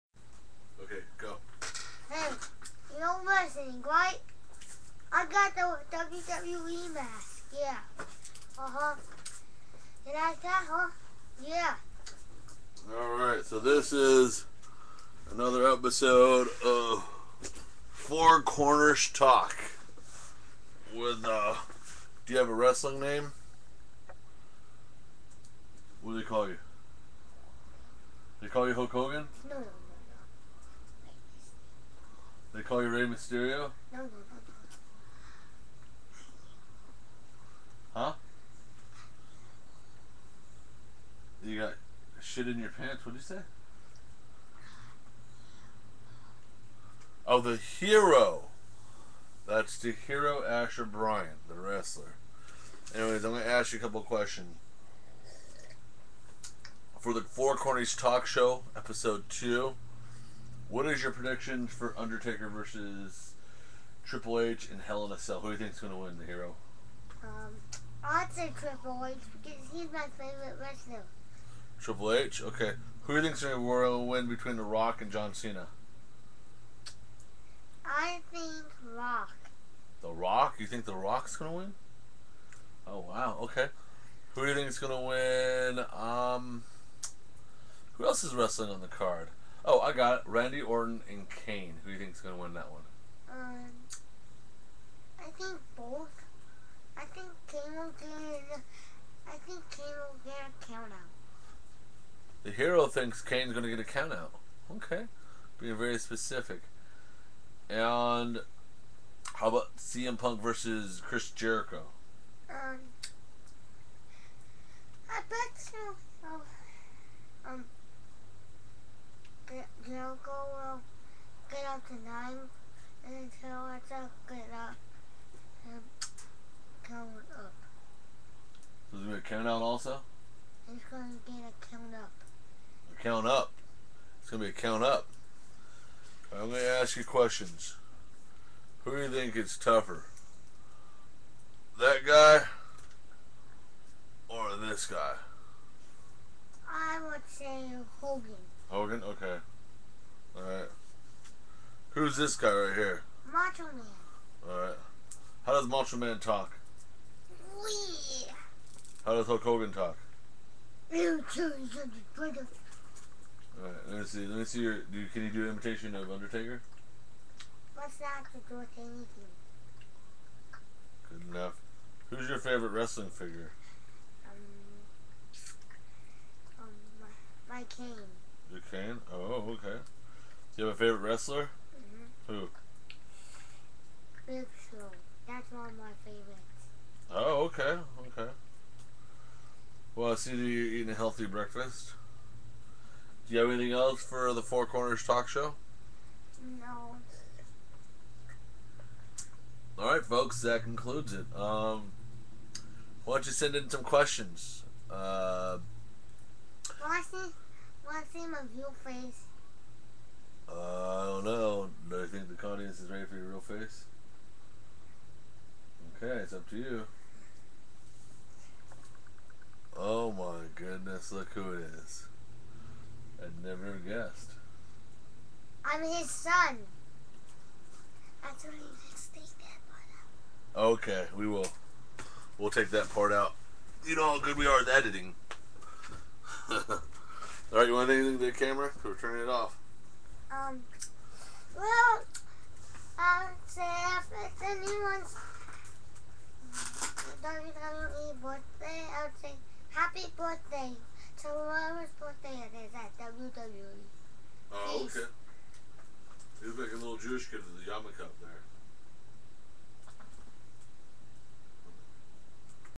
4cornerstalk episode 2dstudentproductions's Webcam Video from March 30, 2012 05:40 AM
predictions about wrestlmania from the masked luchador the hero , the hero reveals he is actualy my son , the hero wishs wwe stars happy bdaydstudentproductions's Webcam Video from March 30, 2012 05:40 AM